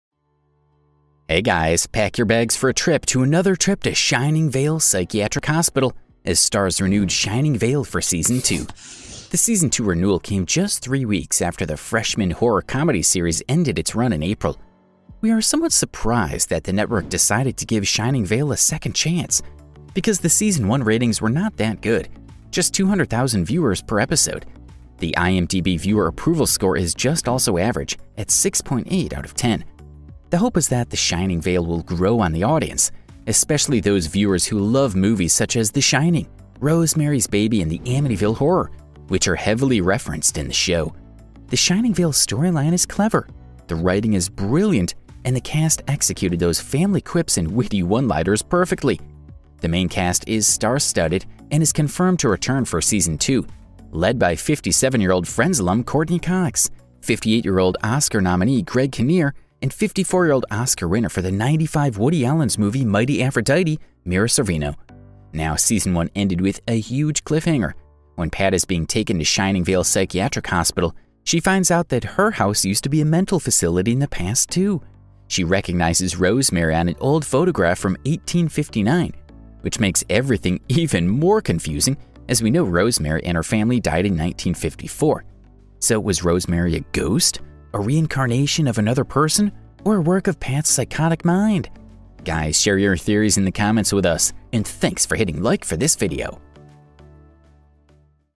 SHINING VALE Season 2 Release Set for 2023. Ending Explained: Is Rosemary a Ghost or Reincarnation?
Hey, guys! Pack your bags for a trip to another trip to Shining Vale psychiatric hospital as Starz renewed Shining Vale for Season 2! The Season 2 renewal came just three weeks after the freshman horror comedy series ended its run in April. We are somewhat surprised that the network decided to give Shining Vale a second chance, because the Season 1 ratings were not that good, just 200 thousand viewers per episode. The IMDb viewer approval score is also just average, at 6.8 out of 10. The hope is that Shining Vale will grow on the audience, especially those viewers who love movies such as The Shining, Rosemary's Baby and The Amityville Horror, which are heavily referenced in the show. The Shining Vale storyline is clever, the writing is brilliant and the cast executed those family quips and witty one-liners perfectly. The main cast is star studded and is confirmed to return for Season 2, led by 57-year old Friends alum Courteney Cox, 58-year old Oscar-nominee Greg Kinnear and 54-year old Oscar-winner for the ’95 Woody Allen's movie Mighty Aphrodite, Mira Sorvino. Now, Season 1 ended with a huge cliffhanger: when Pat is being taken to Shining Vale psychiatric hospital, she finds out that HER house used be a mental facility in the past, too. She recognizes Rosemary on an old photograph from 1859, which makes everything even more confusing as we know Rosemary and her family died in 1954. So, was Rosemary a ghost, a reincarnation of another person or a work of Pat’s psychotic mind? Guys, share your theories in the comments with us.